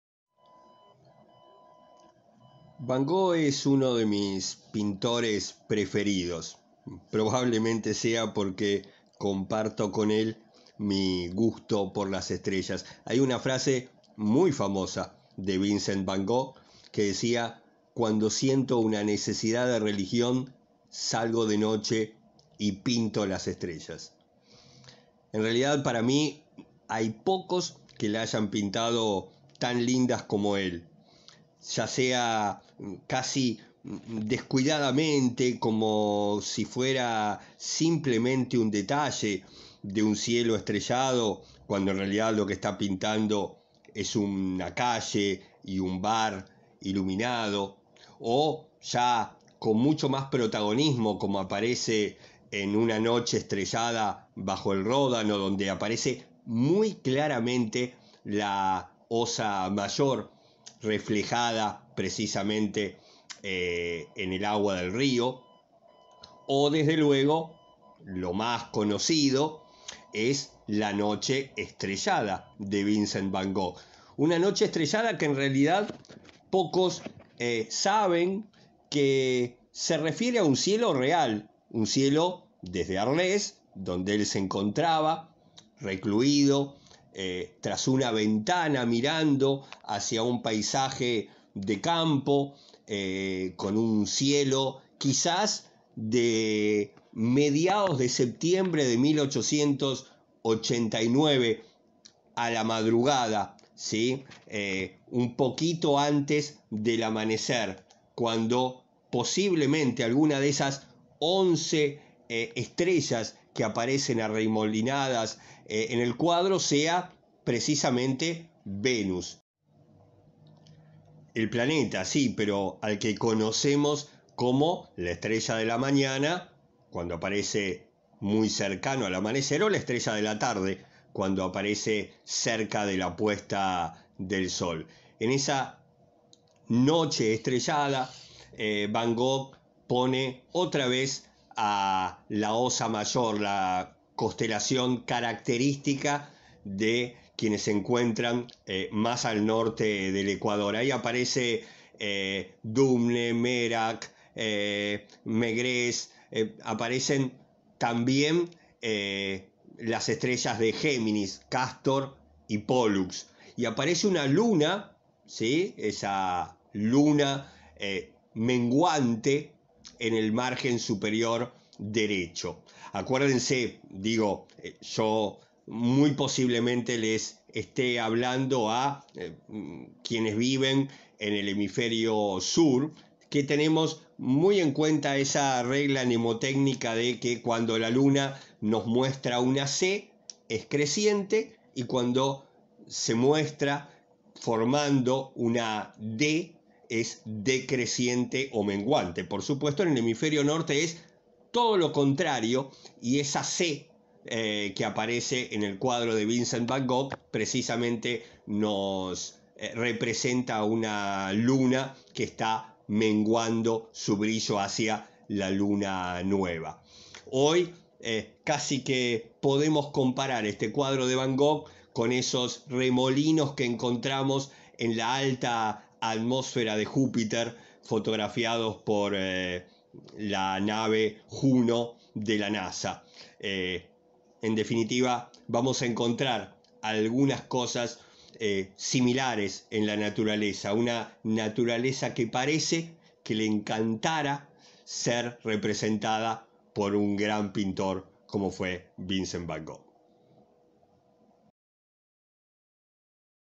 VAN GOGH y la noche estrellada
¿Sabías que el cielo que pintó Van Gogh es real? ¿Y podemos conocer la fecha en que lo miró?